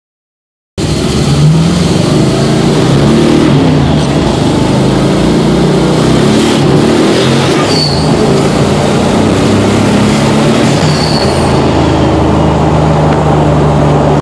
68 Camaro: 572 w/ twin 91s
Fresh out of the shop for it's virgin run on the street. Built by Mayhem Customs